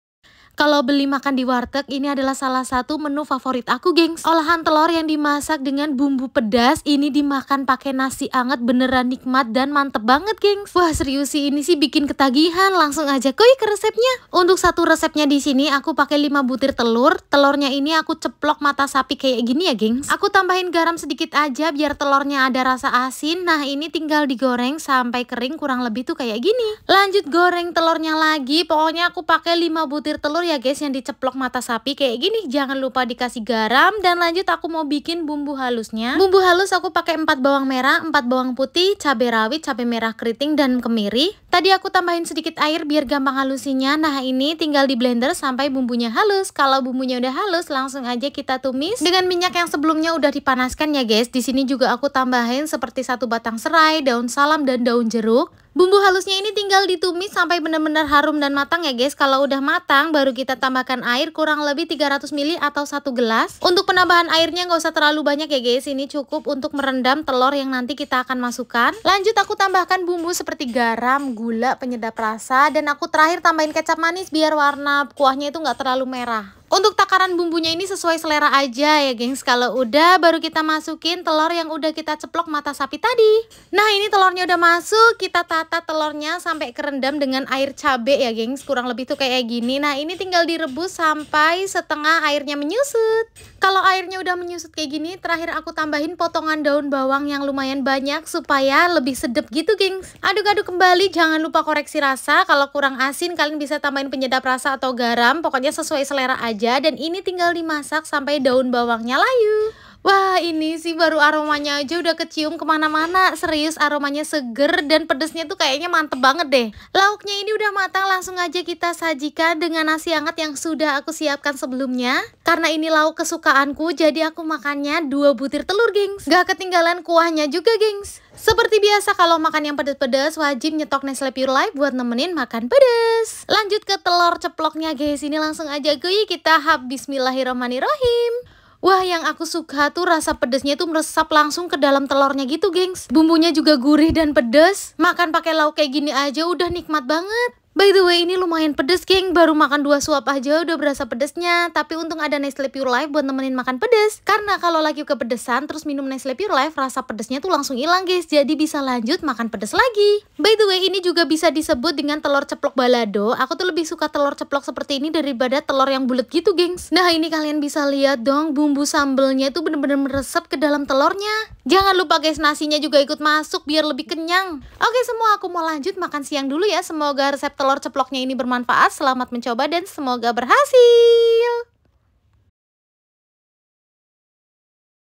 OLAHAN TELOR YANG PALING FAVORIT BANGET KALO LAGI MAMPIR KE WARTEG..!!!!
Menu favorit aku pas kewarteg nih gengs, olahan telor yang dimasak pakai bumbu pedas dan dimakan pakai nasi anget tuh beneran enak dan mantep banget gengs.
Langsung aja kuyy keresepnya

BAHAN :
- 5 butir telur ceplok mata sapi
- 4 Bawang merah
- 4 Bawang putih
- 15 Cabe rawit
- 10 Cabe merah keriting
- 2 kemiri
- Serai
- Daun salam
- Daun jeruk
- 300ml air
- 1/4 sdt garam
- 1/2 sdt gula
- 1/4 sdt penyedap
- 1 sdt kecap manis
- Daun Bawang

Ini sih emang seenak itu gengs, rasa pedesnya juga meresap sampai kedalem telur dan dimakan sama nasi anget tuh nambah plus plus nikmatnya. Pokoknya kalian wajib cobain yaa.